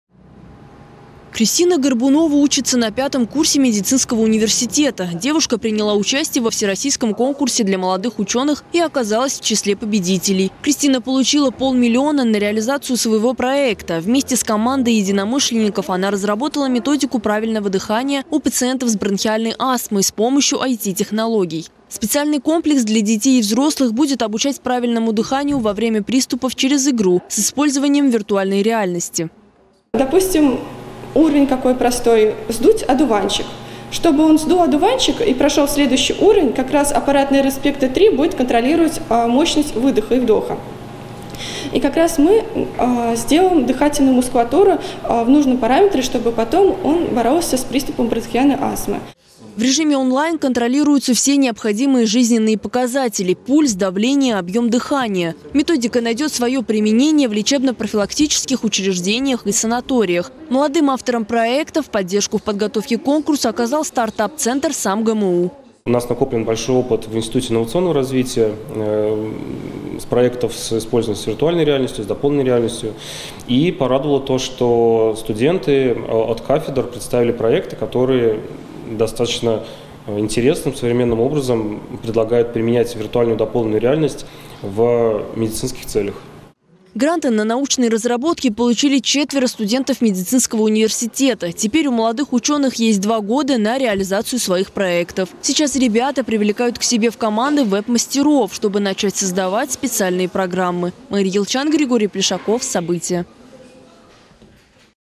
Четыре самарских студента-медика выиграли гранты на реализацию своих проектов
Реабилитация с помощью виртуальной реальности. Студенты СамГМУ выиграли гранты размером по полмиллиона рублей на реализацию своих научных разработок. Молодежь региона все большое вовлекается в инновационную деятельность и делают научные открытия. Об одной из таких проектов расскажет Мери Елчян.